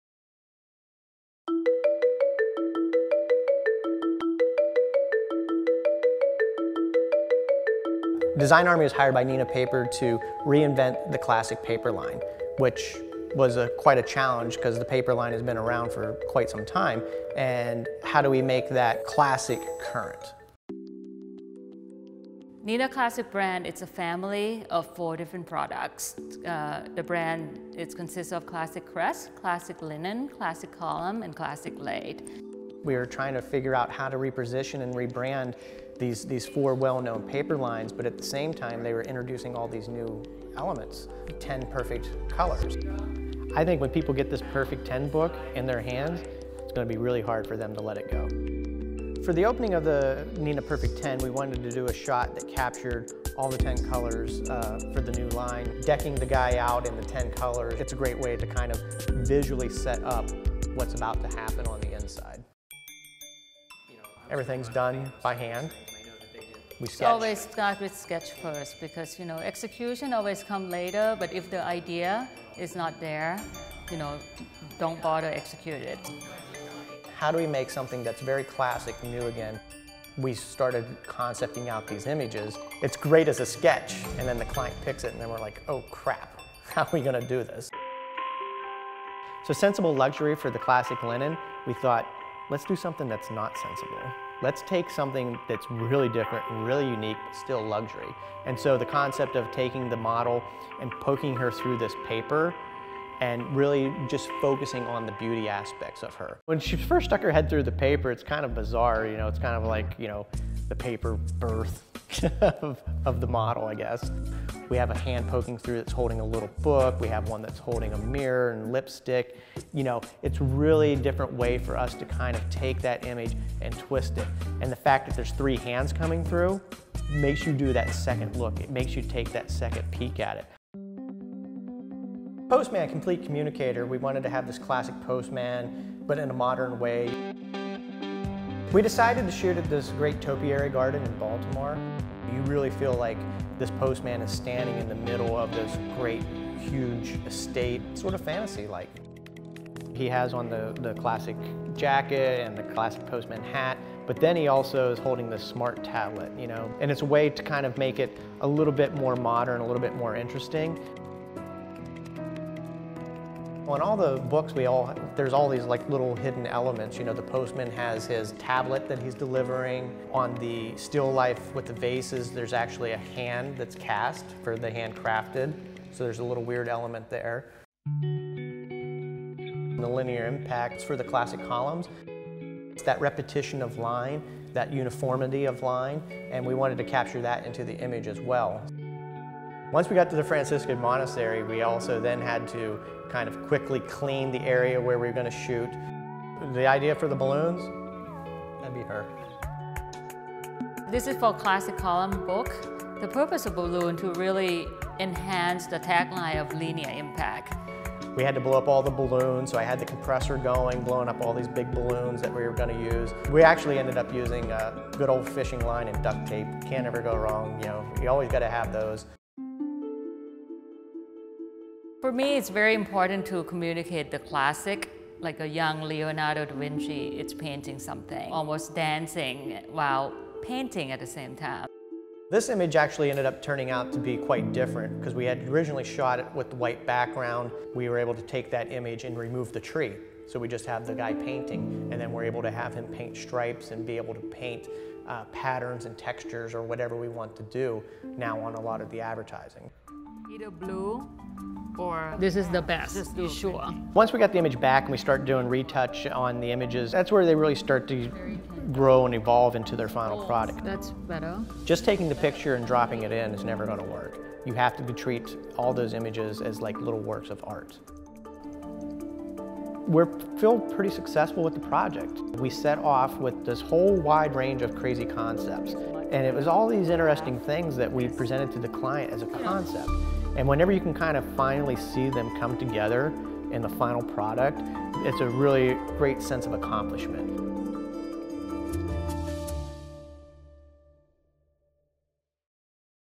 CLASSIC® Papers Revision by Design Army: Behind the Scenes
Design Army out of Washington, DC redesigns the CLASSIC® Papers Brands for Neenah Paper. See their vision and the wonderful behind the scenes look at the task of revising a classic.